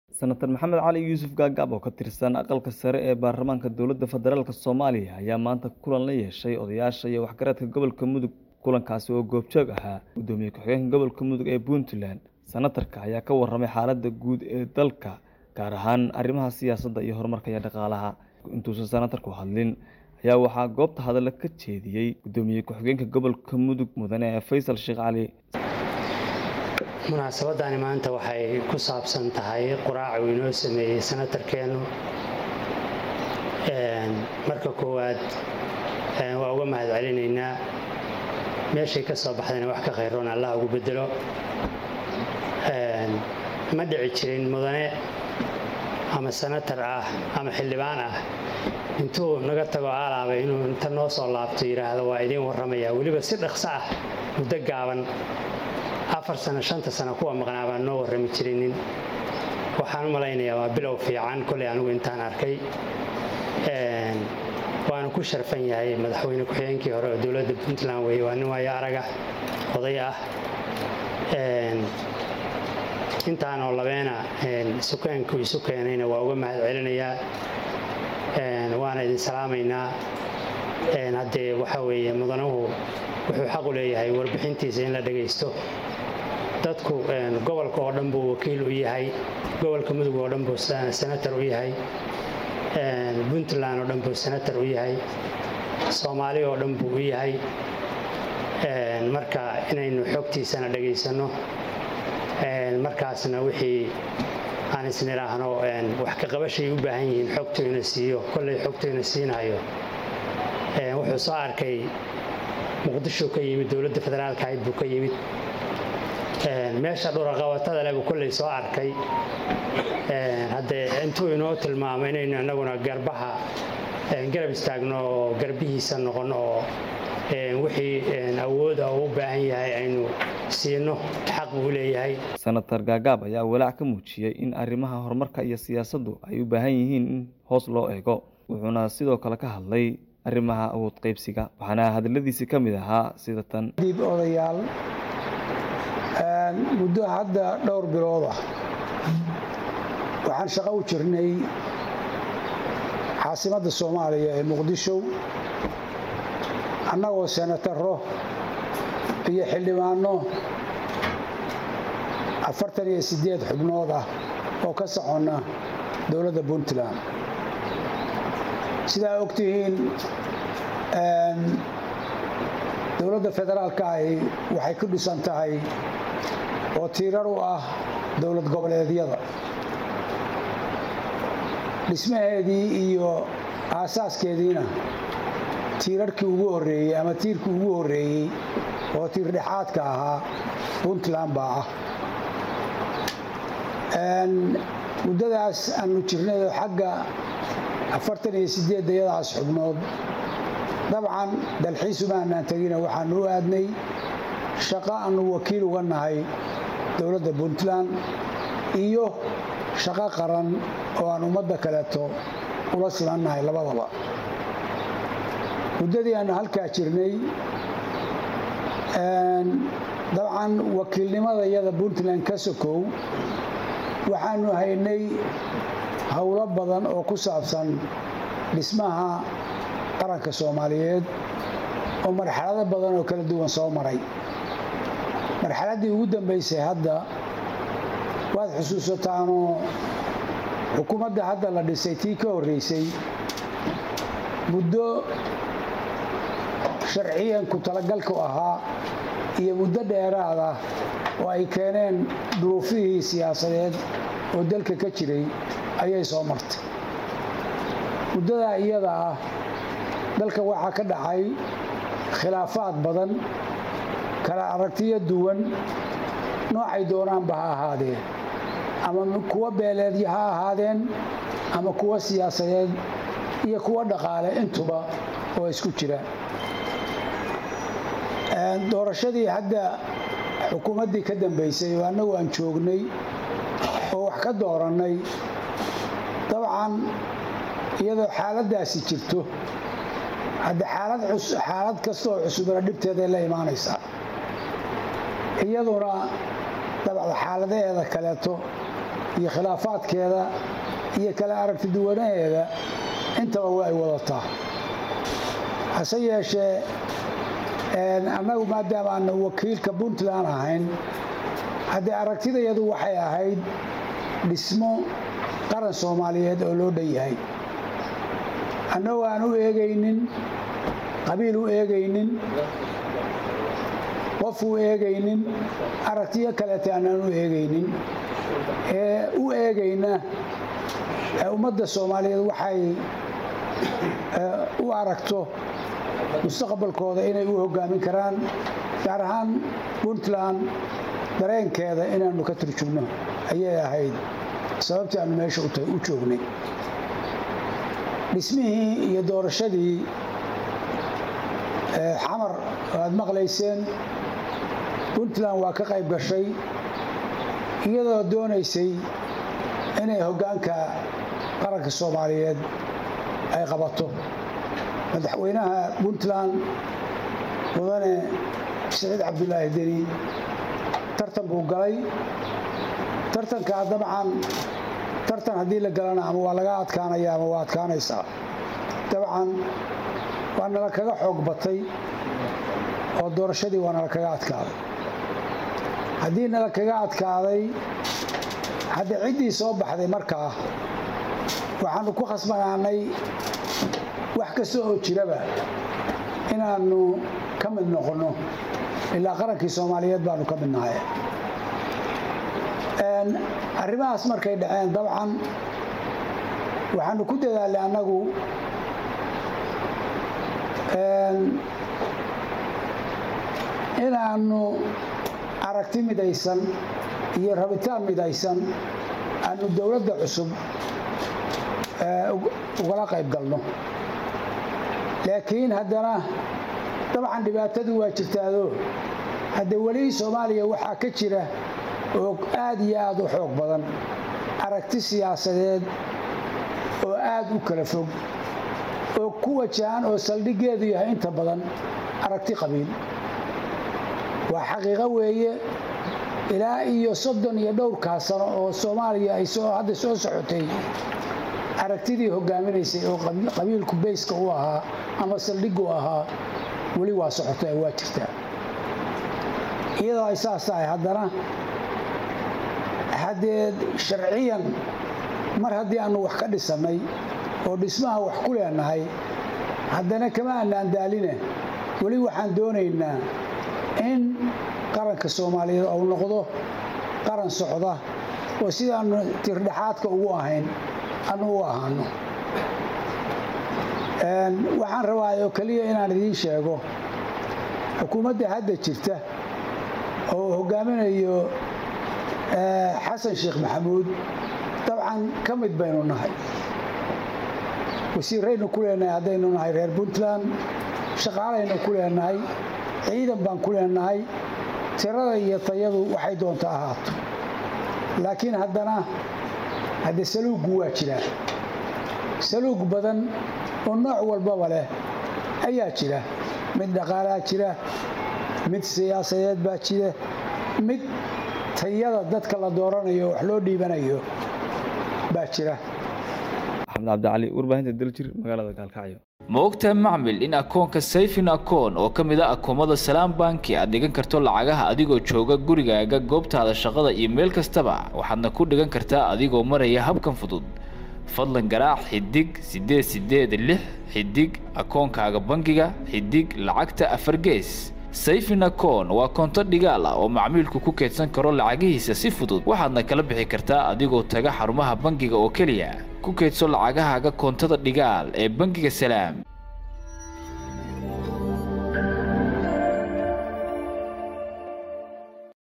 Senator Gaagaab oo dhaliilay Madaxweyne Xasan Shiikh iyo caddaalad darro horleh oo bilaabatay
Senator Gaagaab, PUNTLAND: “Xukuumadda Xasan Shiikh xilalkii muhiimka ahaa waxay ku saftay beesha uu ka soojeedo. Puntland lama siin wax u qalma. Reer Puntland waxaan u sheegayaa Xamar waxaan magac ahayn in aysan naga jirin"